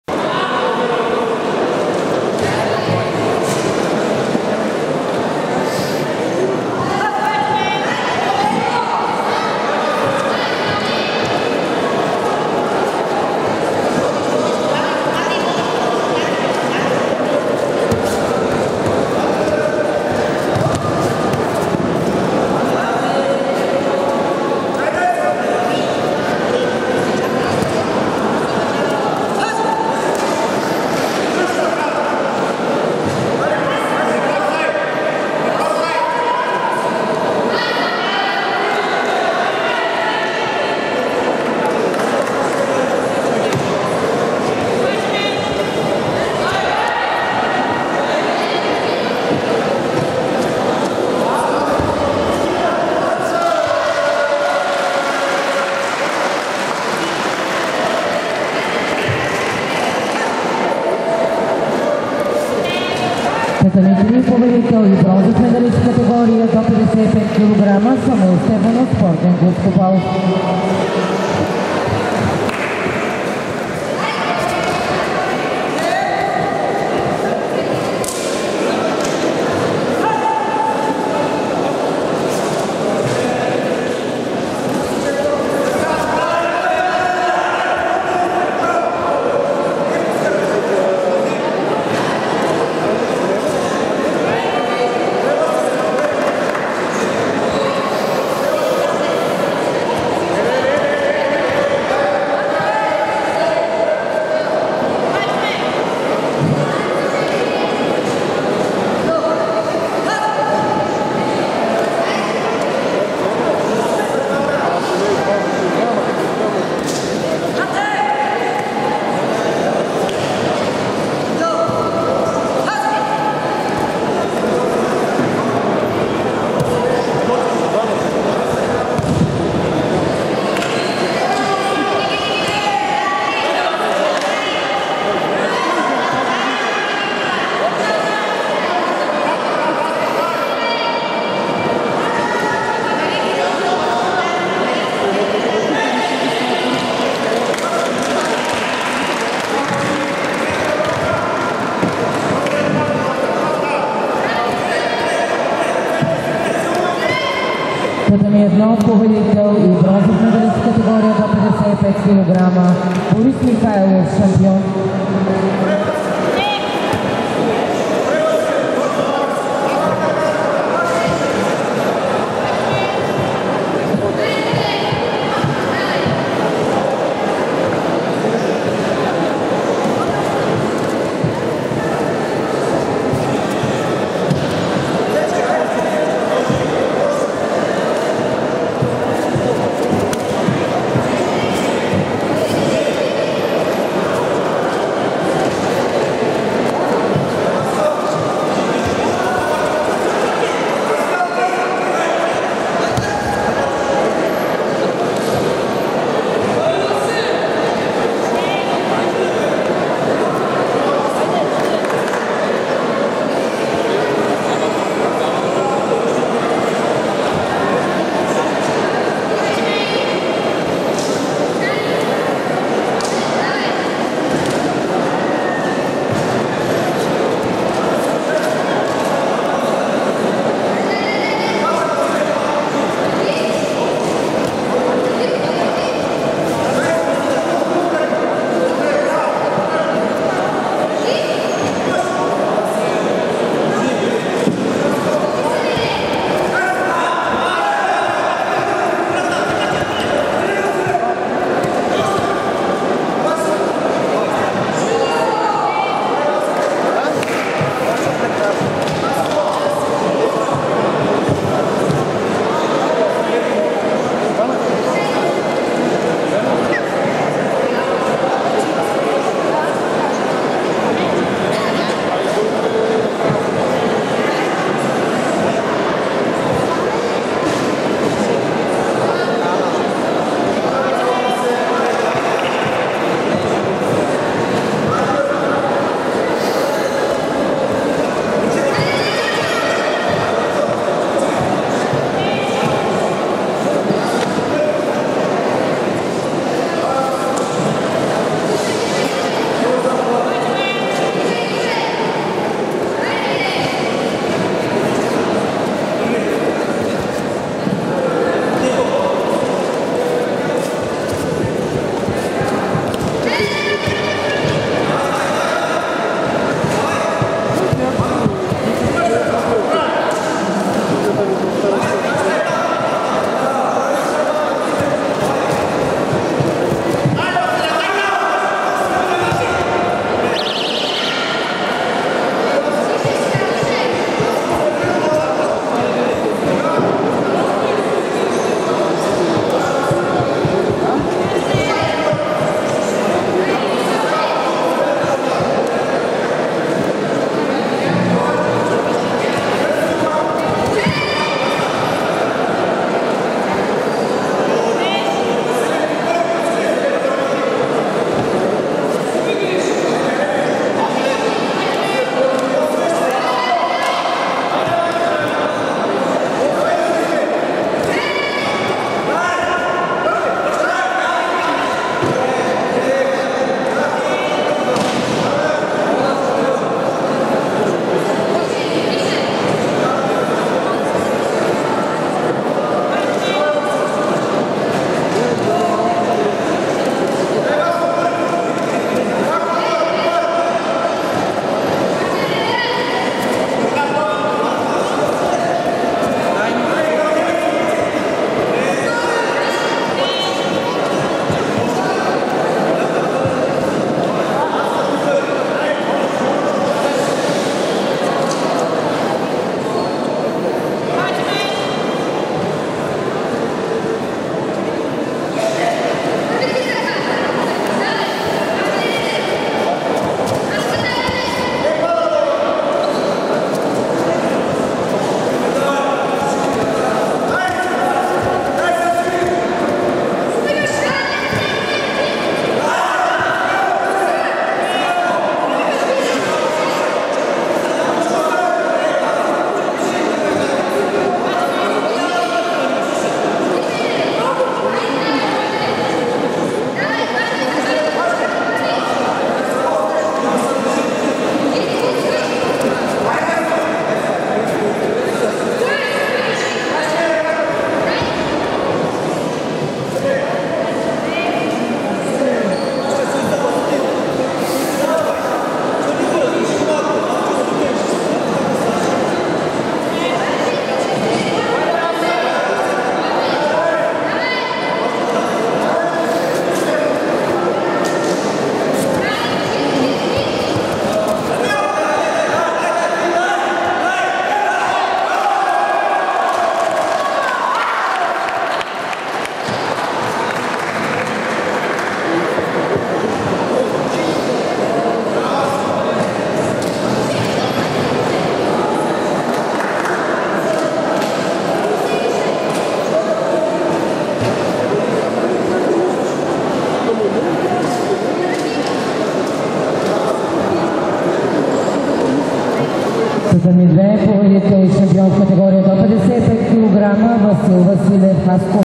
Vasil VASILEV vs Velimir POPOV | Judo Championship Bulgaria U18 Panagyurishte 2023 FINAL 55 kg
Judo Championship Bulgaria U18 Panagyurishte 2023
FINAL 55 kg
Vasil VASILEV vs  Velimir POPOV